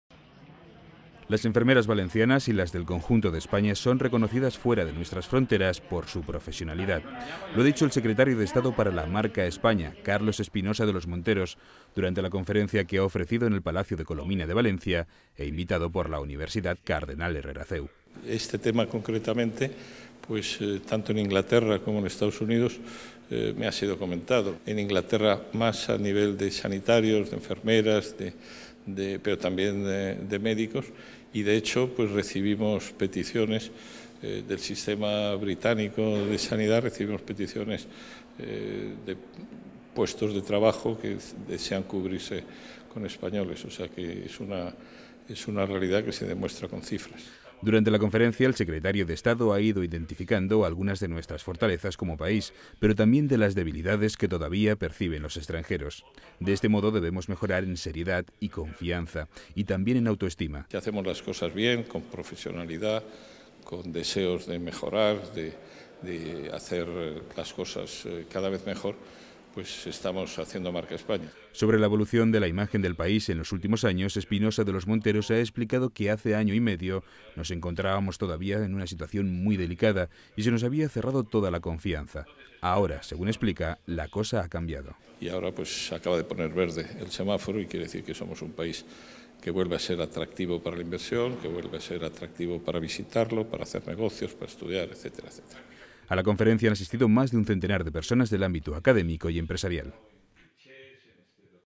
Conferencia sobre la Marca España | Cecova TV
La Universidad Cardenal Herrera CEU ha organizado en el Palacio de Colomina de Valencia una conferencia sobre la Marca España bajo el título "Una empresa de todos". El Secretario de Estado para la Marca, Carlos Espinosa de los Monteros, ha descrito la evolución de la iniciativa durante el último año y medio. Un conferencia en la que hemos conocido como la Enfermería también ayuda a mejorar la imagen de nuestro país.